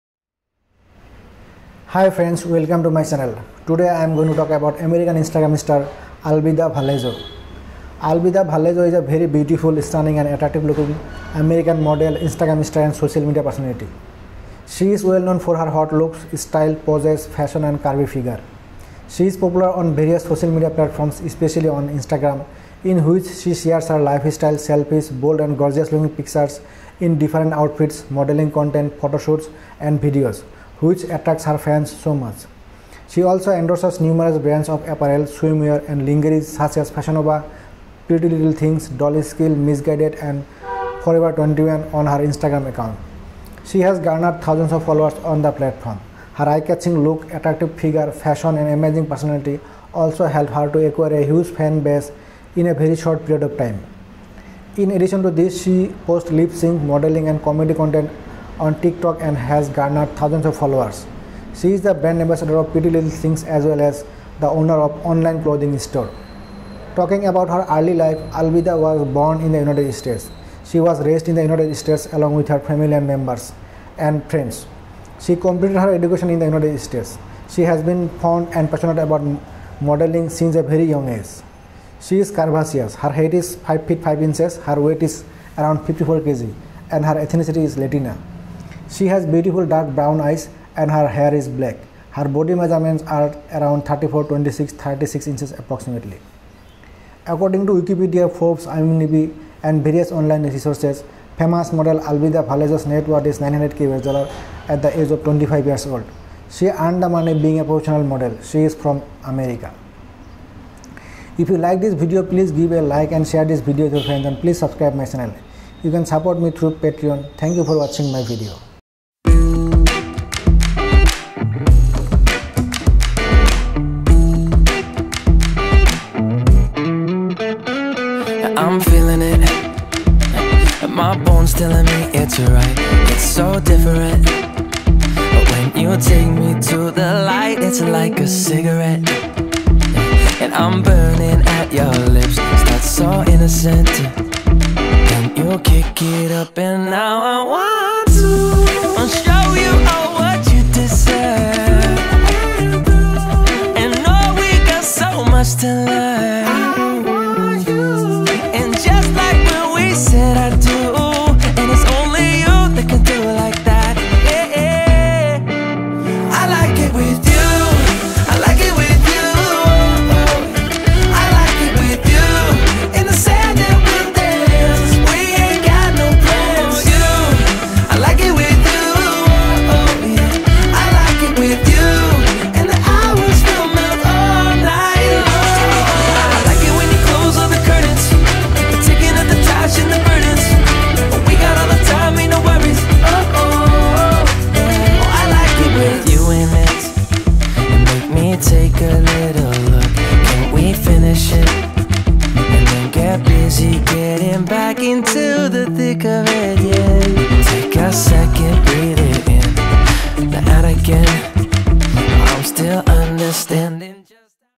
American Instagram Star Albida Vallejo| Wiki| Home| Life Style| Body Measurements| Biography
Albida Vallejo is a very beautiful, stunning, and attractive-looking American model, Instagram star, and social media personality. She is well known for her hot looks, style, poses, fashion, and curvy figure. She is popular on various social media platforms, especially on Instagram in which she shares her lifestyle, selfies, bold and gorgeous looking pictures in different outfits, modeling content, photoshoots, and videos which attracts her fans so much. She also endorses numerous brands of apparel, swimwear, and lingerie such as Fahion Nova, Pretty Little Things, Doll Skill, Miss Guided, and Forever21 on her Instagram account.